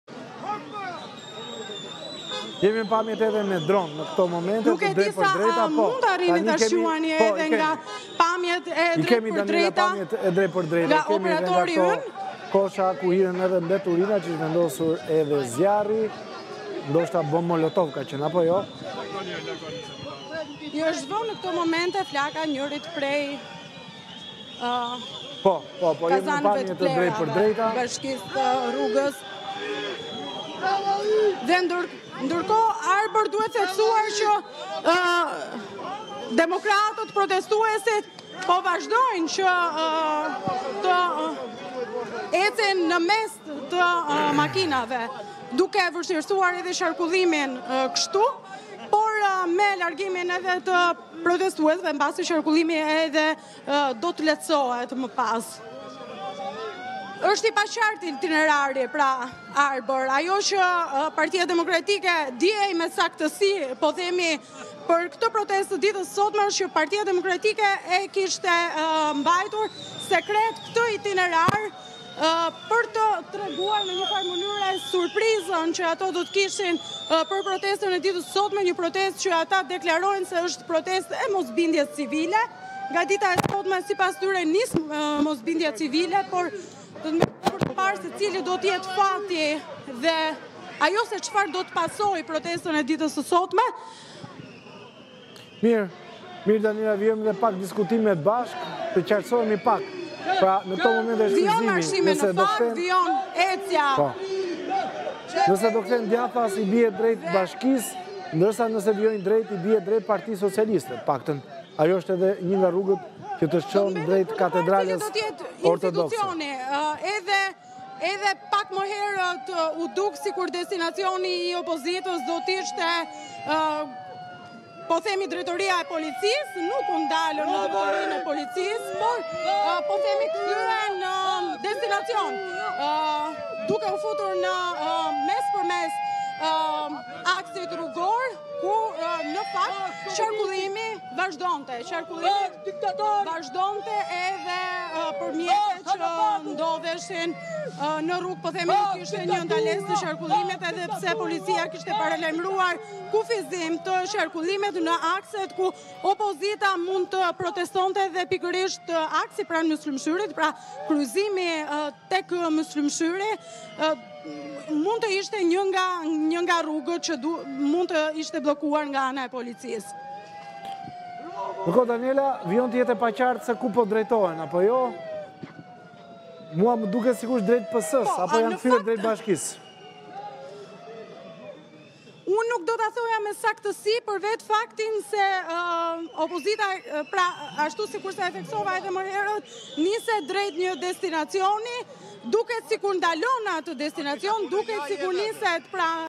Pas stacionit të autobusit, protestuesit i vënë flakën kazanit të plehrave
Protestuesit e opozitës u larguan nga kryeministria të udhëhequr nga Flamur Noka dhe Gazment Bardhi.

Teksa po kalonin nga Tajvani disa protestues i vunë flakën një kazani plehrash për të treguar mosbindjen civile.